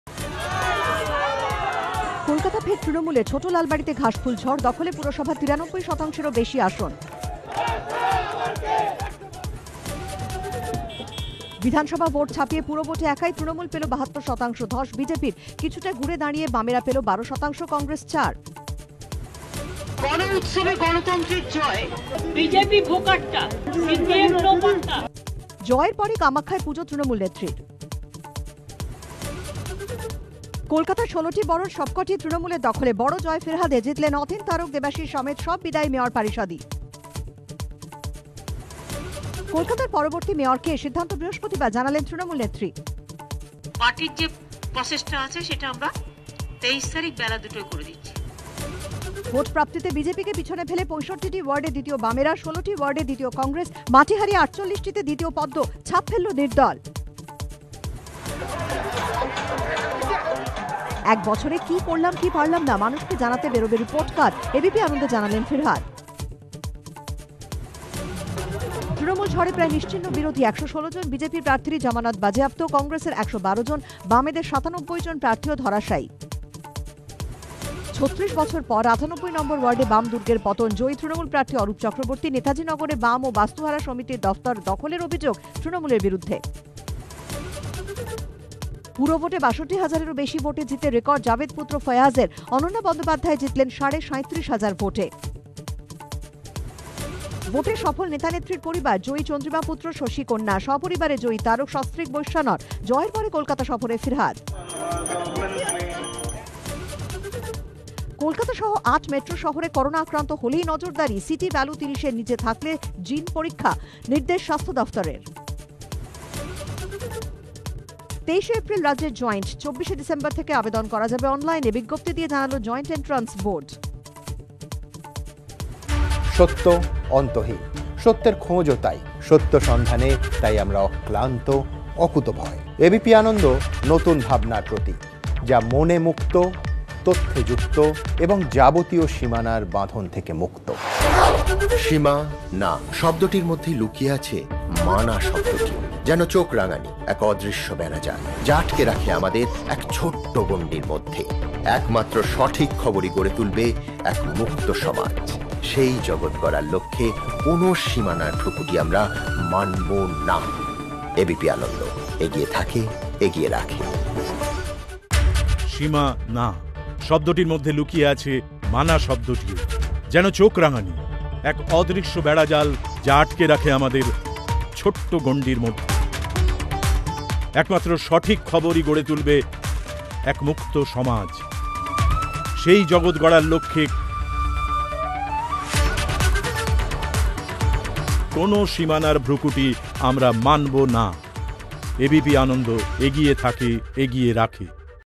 Morning Headlines: কলকাতা ফের তৃণমূলের, ছোট লালবাড়িতে ঘাসফুল ঝড় | Bangla News
কলকাতা ফের তৃণমূলের (TMC)। ছোট লালবাড়িতে ঘাসফুল ঝড়। দখলে পুরসভা, ৯৩ শতাংশেরও বেশি আসন।
বিধানসভা ভোট ছাপিয়ে পুরভোটে একাই তৃণমূল পেল ৭২ শতাংশ। ধস বিজেপির (BJP)। কিছুটা ঘুরে দাঁড়িয়ে বামেরা পেল ১২ শতাংশ। কংগ্রেস চার। জয়ের পরই কামাখ্যায় পুজো তৃণমূল নেত্রীর (Mamata Banerjee)।
কলকাতার ১৬টি বরোর সবকটিই তৃণমূলের দখলে। বড় জয় ফিরহাদ হাকিমের (Firhad Hakim)। জিতলেন অতীন ঘোষ, তারক সিনহা (Tarak Sinha), দেবাশিস কুমার-সহ সব বিদায়ী মেয়র পারিষদই।
কলকাতার পরবর্তী মেয়র কে? সিদ্ধান্ত বৃহস্পতিবার, জানালেন তৃণমূল নেত্রী।
ভোটপ্রাপ্তিতে বিজেপিকে পিছনে ফেলে ৬৫টি ওয়ার্ডে দ্বিতীয় বামেরা। ১৬টি ওয়ার্ডে দ্বিতীয় কংগ্রেস। মাটি হারিয়ে ৪৮টিতে দ্বিতীয় পদ্ম। ছাপ ফেলল নির্দল।
একবছরে কী করলাম, কী পারলাম না - মানুষকে জানাতে বেরোবে রিপোর্ট-কার্ড। এবিপি আনন্দে জানালেন ফিরহাদ হাকিম।
তৃণমূল ঝড়ে প্রায় নিশ্চিহ্ন বিজেপি, ১১৬ জন বিজেপি প্রার্থীরই জমানত বাজেয়াপ্ত, কংগ্রেসের (Congress) ১১২ জন, বামেদের ৯৭ জন প্রার্থীও ধরাশায়ী।
৩৬ বছর পর ৯৮ নং ওয়ার্ডে বাম-দুর্গের পতন। জয়ী তৃণমূল প্রার্থী অরূপ চক্রবর্তী (Arup Chakraborty)। নেতাজিনগরে বাম ও বাস্তুহারা সমিতির দফতর দখলের অভিযোগ তৃণমূলের বিরুদ্ধে।
ভোটে সফল নেতানেত্রীর পরিবার। জয়ী চন্দ্রিমা-পুত্র, শশী-কন্যা। সপরিবারে জয়ী তারক সিনহা, সস্ত্রীক বৈশ্বানর। জয়ের পরে কলকাতা সফরে ফিরহাদ।
হলদিয়া (Haldia) আইওসি প্লান্টে বিধ্বংসী আগুন। তিনজনের মৃত্যু। অগ্নিদগ্ধ ৪৪। গ্রিন করিডোরে ২৮ জনকে আনা হল কলকাতায়। তদন্তের নির্দেশ পেট্রোলিয়াম মন্ত্রীর।
কলকাতা-সহ ৮ মেট্রো শহরে করোনা আক্রান্ত হলেই নজরদারি। সিটি ভ্যালু ৩০-র নীচে থাকলেই জিন পরীক্ষা, নির্দেশ স্বাস্থ্য দফতরের।
২৩ এপ্রিল রাজ্যের জয়েন্ট। ২৪ ডিসেম্বর থেকে আবেদন করা যাবে অনলাইনে, বিজ্ঞপ্তি দিয়ে জানাল জয়েন্ট এন্ট্রান্স বোর্ড।
দেশে ওমিক্রন (Omicron) আক্রান্ত ২০০ পার। স্থানীয় স্তরে কনটেনমেন্টে জোর দিতে নির্দেশ কেন্দ্রের। মার্কিন মুলুকে ওমিক্রনে প্রথম মৃত্যু।
আইএসএলের (ISL) ফিরতি ডার্বি ২৯ জানুয়ারি। ফতোরদায় মুখোমুখি এটিকে মোহনবাগান ও এসসি ইস্টবেঙ্গল। দ্বিতীয় লেগের ক্রীড়াসূচি প্রকাশ।ফিরতি ডার্বি ২৯ জানুয়ারি